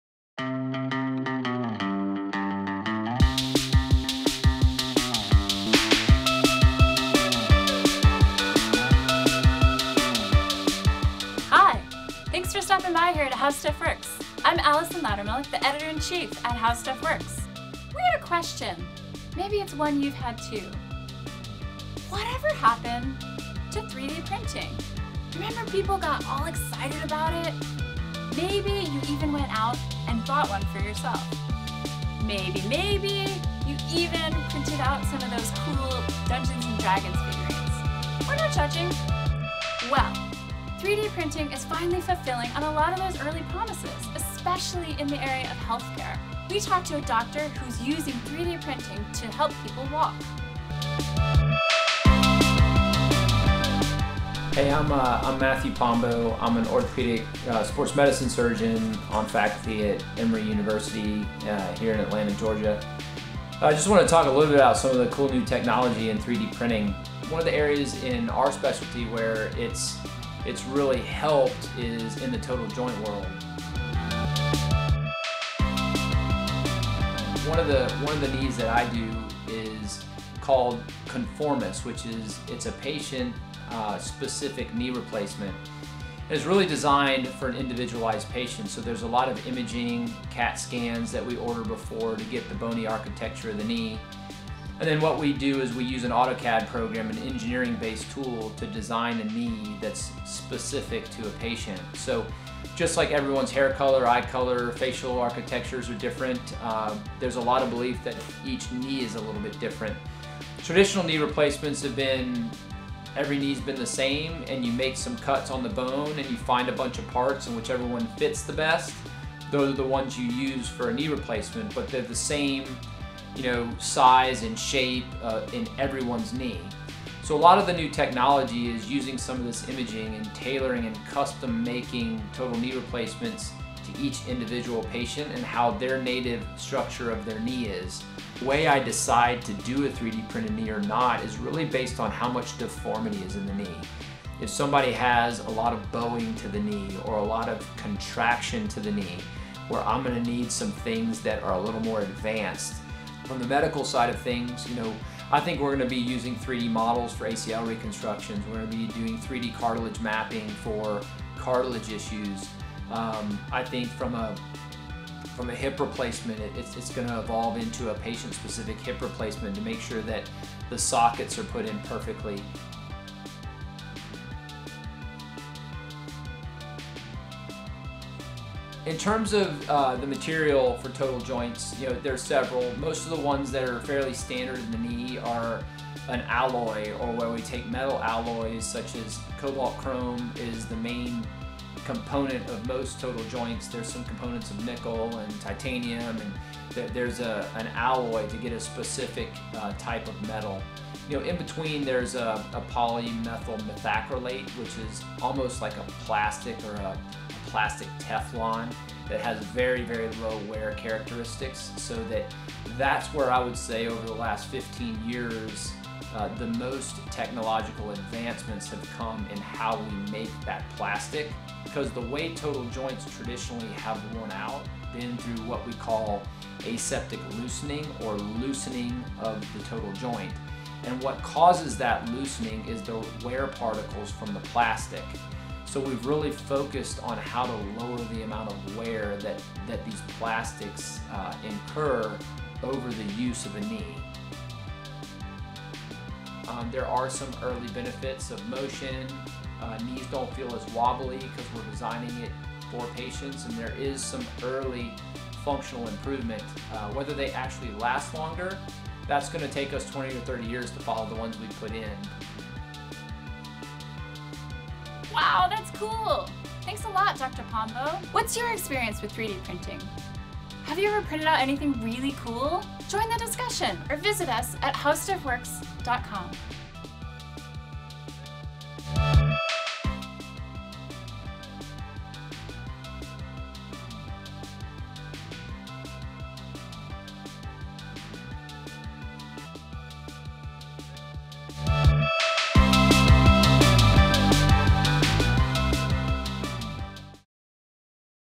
Can 3-D Printing Help People Walk?
HowStuffWorks interviews Dr. Mathew Pombo, an Emory
Orthopedic Sports Medicine Surgeon, about how 3D-printing is used to help people walk.